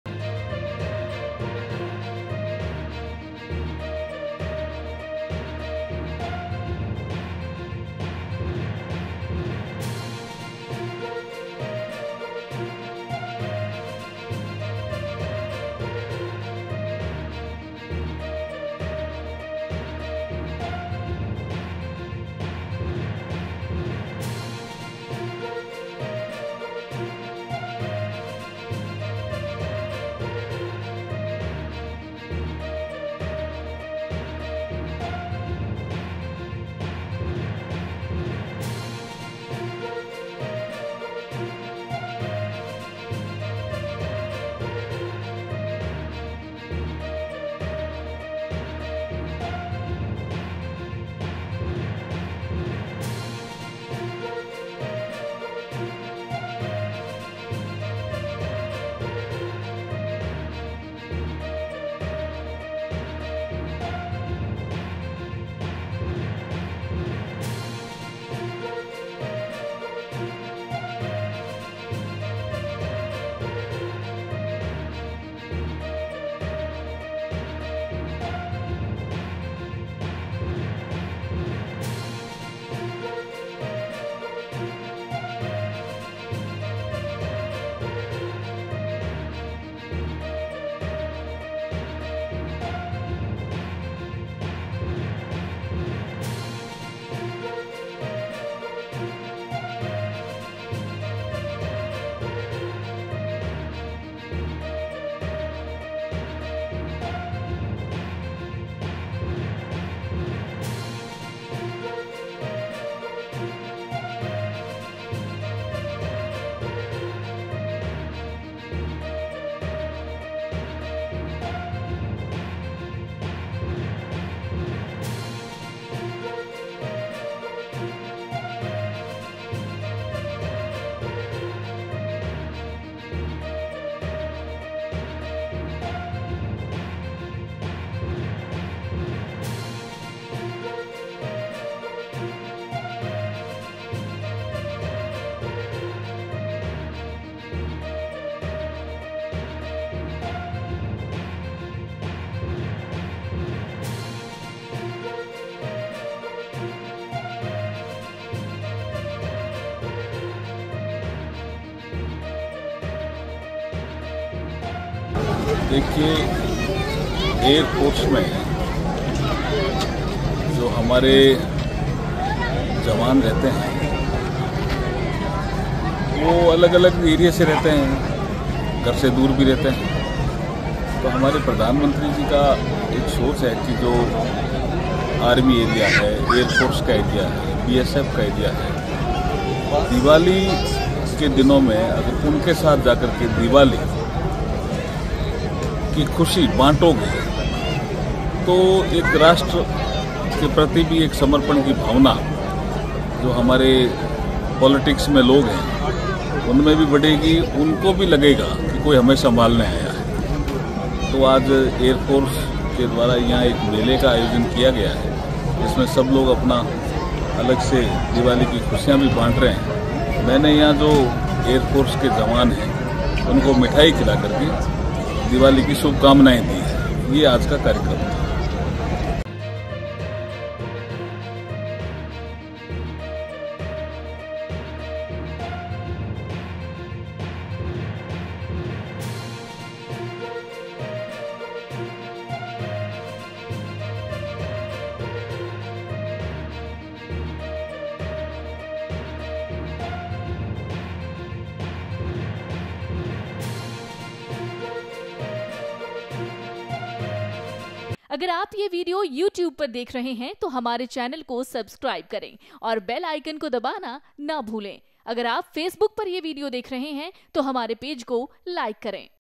Rajasthan News : जवानों के साथ दिवाली मनाते हुए Arjun Ram Meghwal का खास वीडियो | Diwali 2024
केंद्रीय कानून मंत्री अर्जुन राम मेघवाल ने नाल एयर फोर्स स्टेशन पर जवानों के साथ दिवाली मनाई।